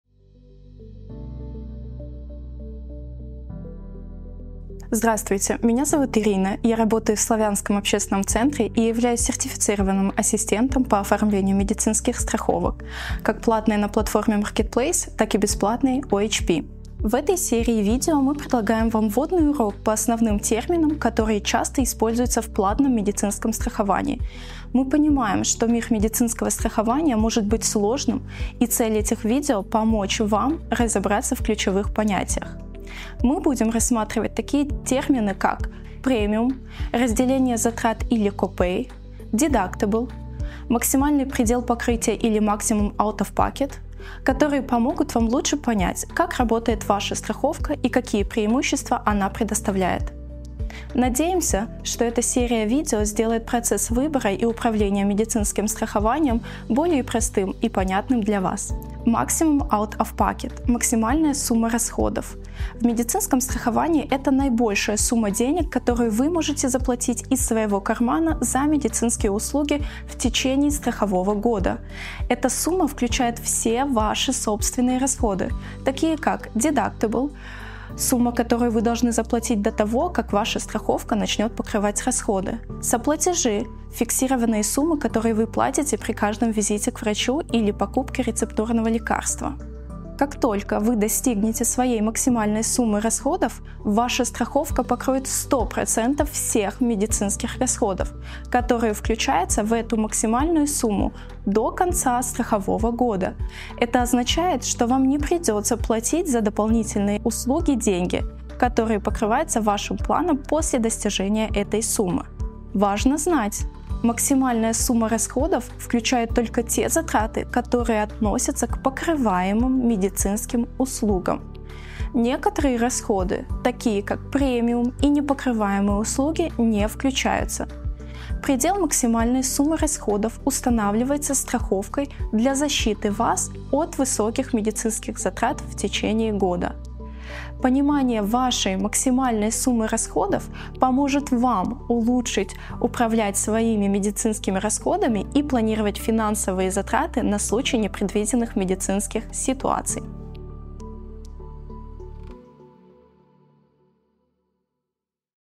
Максимум out-of-pocket: Как защитить себя от высоких медицинских расходов?
💡 Что такое максимум out-of-pocket в медицинском страховании? Это максимальная сумма, которую вы можете заплатить из своего кармана за медицинские услуги в течение года. 🏥 Как только вы достигнете этого лимита, страховка покроет 100% ваших медицинских расходов до конца страхового года. Это включает дедактабл и соплатежи за визиты к врачу и лекарства.

Важно помнить, что премиумы и непокрываемые услуги не входят в этот лимит. Понимание вашего максимума расходов поможет вам лучше управлять медицинскими затратами и подготовиться к неожиданным ситуациям.

Если вам нужна помощь — специалисты Славянского общественного центра всегда готовы поддержать вас!